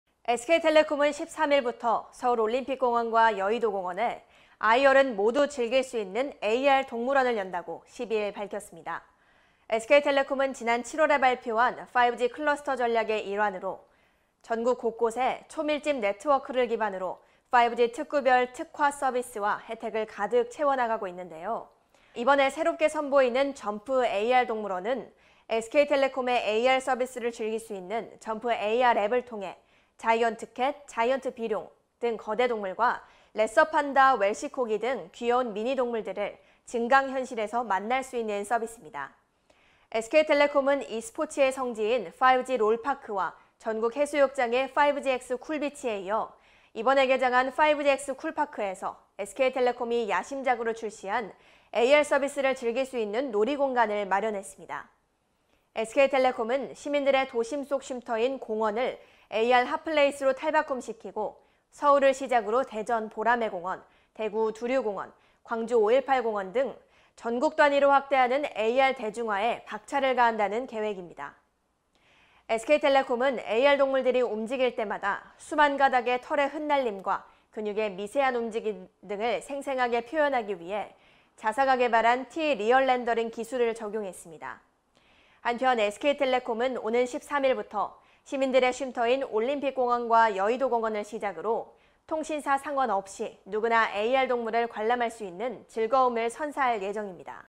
[라이브썰] SK텔레콤, ‘AR동물원’ 연다 … “진짜보다 더 진짜 같네”
SK텔레콤은 13일부터 서울 올림픽공원과 여의도공원에 아이·어른 모두 즐길 수 있는 ‘AR동물원’을 연다고 12일 밝혔습니다.

SK텔레콤은 지난 7월에 발표한 『5G 클러스터』 전략의 일환으로 전국 곳곳에 초밀집 네트워크를 기반으로 5G 특구 별 특화 서비스와 혜택을 가득 채워나가고 있는데요.

이번에 새롭게 선보이는 ‘Jump AR동물원’은 SK텔레콤의 AR 서비스를 즐길 수 있는 ‘Jump AR’ 앱을 통해 ‘자이언트 캣’, ‘자이언트 비룡’ 등 거대 동물과 ‘레서판다’, ‘웰시코기’ 등 귀여운 미니동물들을 증강현실에서 만날 수 있는 서비스입니다.

SK텔레콤은 e스포츠의 성지인 ‘5G 롤파크’와 전국 해수욕장의 ‘5GX 쿨비치’에 이어, 이번에 개장한 ‘5GX 쿨파크’에서 SK텔레콤이 야심작으로 출시한 AR 서비스를 즐길 수 있는 놀이공간을 마련했습니다.

SK텔레콤은 시민들의 도심 속 쉼터인 공원을 ‘AR 핫플레이스’로 탈바꿈시키고, 서울을 시작으로 대전 보라매공원, 대구 두류공원, 광주 5.18공원 등 전국 단위로 확대하는 등 AR 대중화에 박차를 가한다는 계획입니다.

SK텔레콤은 AR동물들이 움직일 때마다 수만 가닥의 털의 흩날림과 근육의 미세한 움직임 등을 생생하게 표현하기 위해 자사가 개발한 ‘T 리얼 렌더링 기술’을 적용했습니다.

한편 SK텔레콤은 오는 13일부터 시민들의 쉼터인 올림픽공원과 여의도공원을 시작으로 통신사 상관없이 누구나 AR동물을 관람할 수 있는 즐거움을 선사할 예정입니다.

[진행 = 홍수연 아나운서]